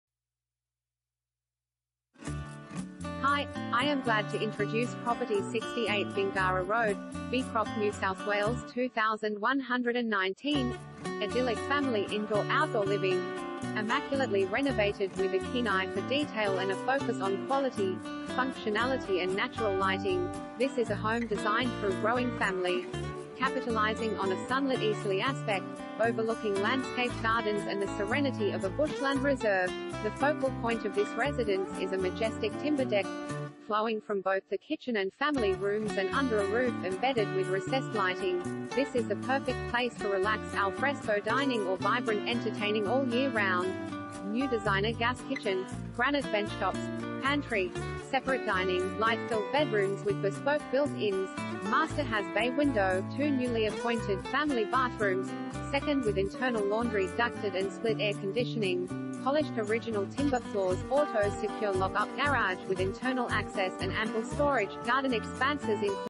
68 Bingara Road, Beecroft NSW 2119 | AuPropertyReport.Com
68 Bingara Road, Beecroft NSW 2119
Property Type: house
Bedrooms: 3
Bathrooms: 3
Car Space: 1
Idyllic family indoor/outdoor living